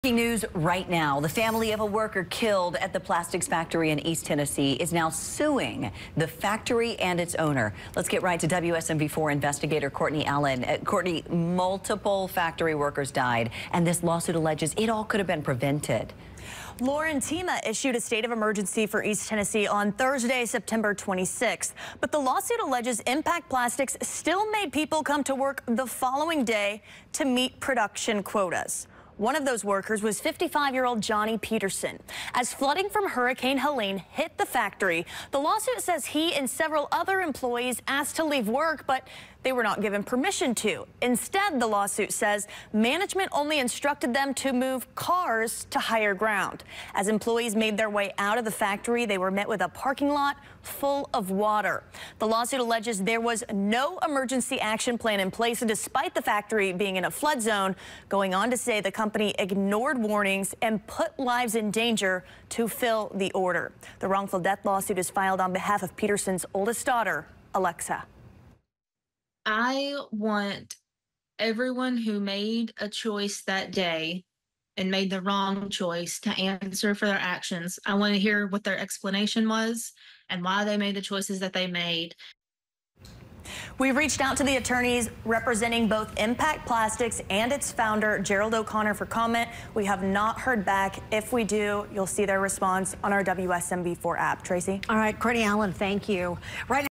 Family of worker suing Impact Plastics
he family of a worker killed at the plastics factory in East Tennessee is now suing the factory and its founder, Gerald O’Connor.